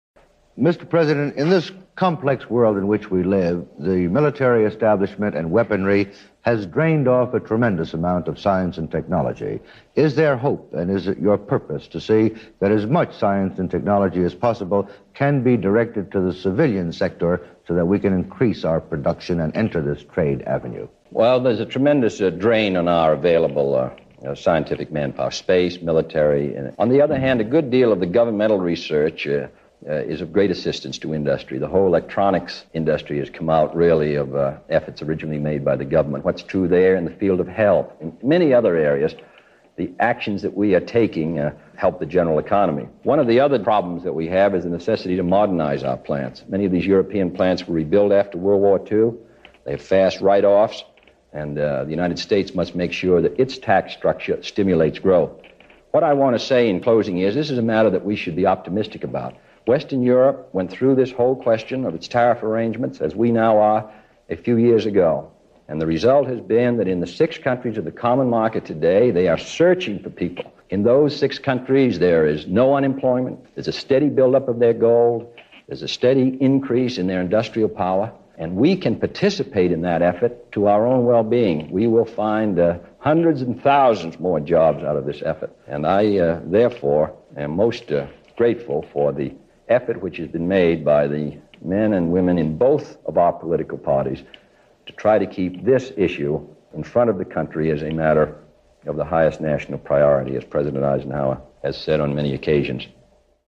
May 11, 1962 - JFK White House Interview with CBS's David Schoenbrun (Clip No. 2)
May 11, 1962 - Clip No. 2 from an interview of President Kennedy today by CBS correspondent David Schoenbrun concerning the economic, political, and social aspects of the Trade Expansion Act of 1962.